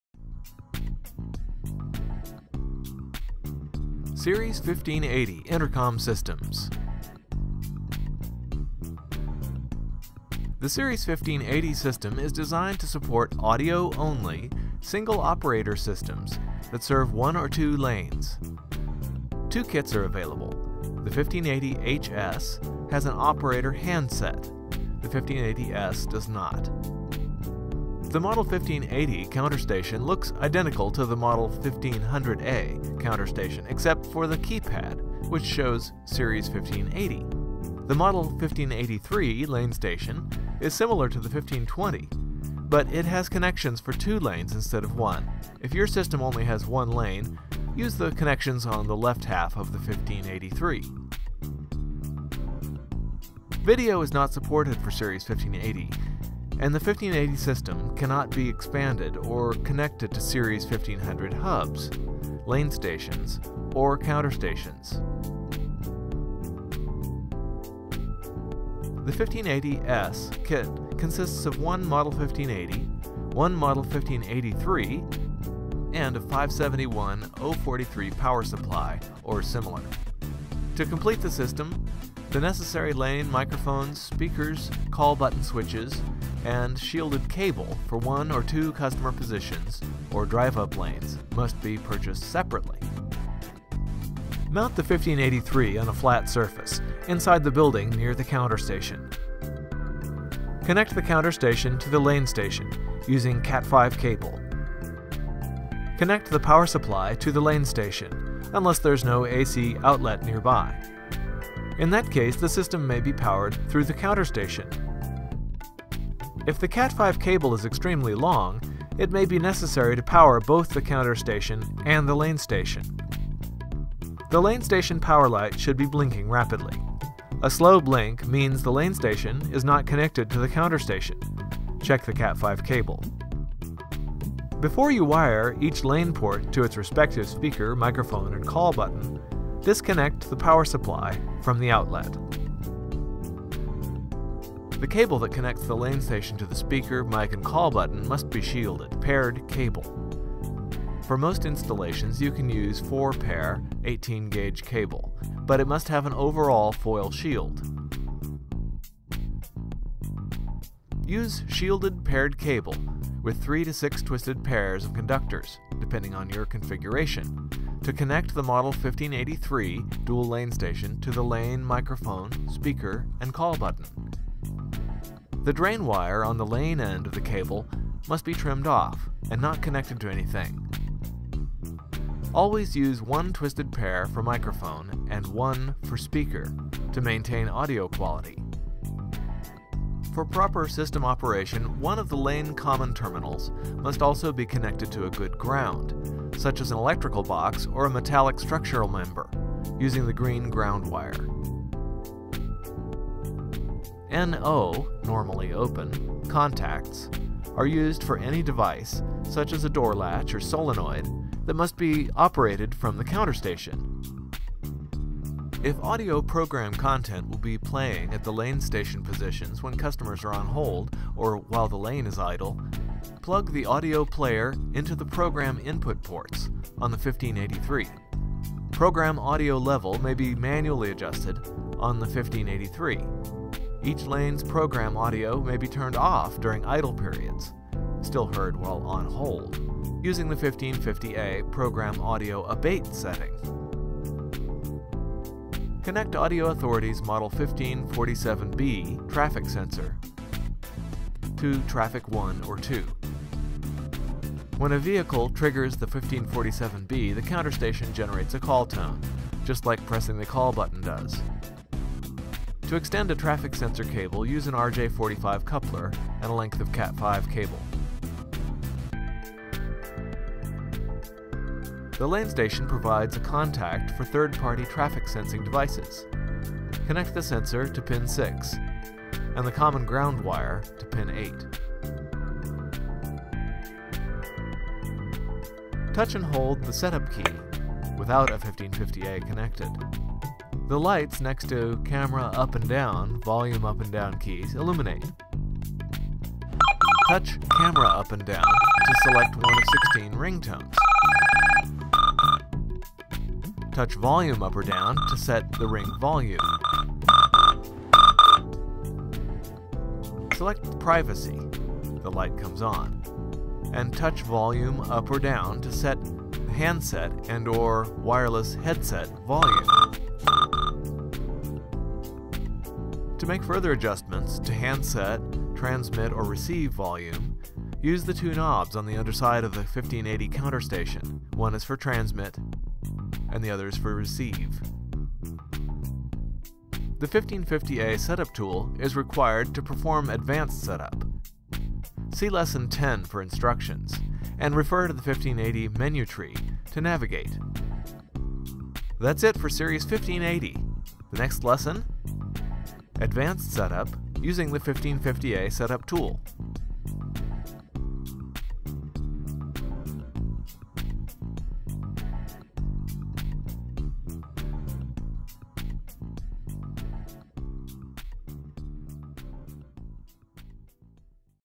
Series 1500 Training Lesson 9
Series 1580 is a special intercom system designed for small, audio-only installations. While similar to Series 1500 in appearance and operation, the 1580 series is quite different.  Lesson 9 covers the unique wiring and installation details for 1580 systems.